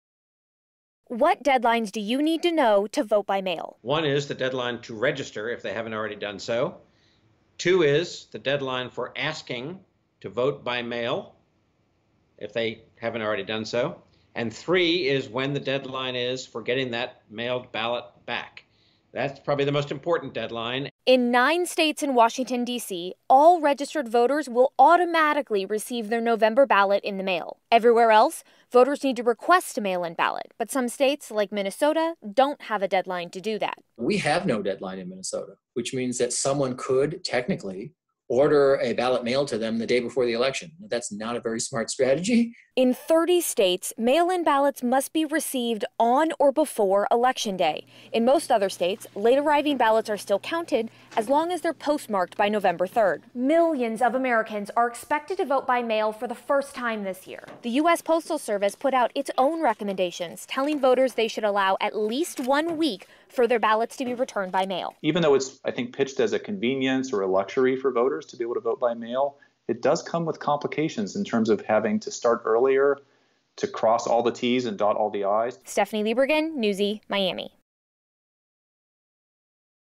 Vote Smarter 2020: Key Vote-By-Mail Deadlines You Need To Know
Newsy's Vote Smarter 2020 aims to provide you with all the information you'll need to safely and successfully cast your ballot this year.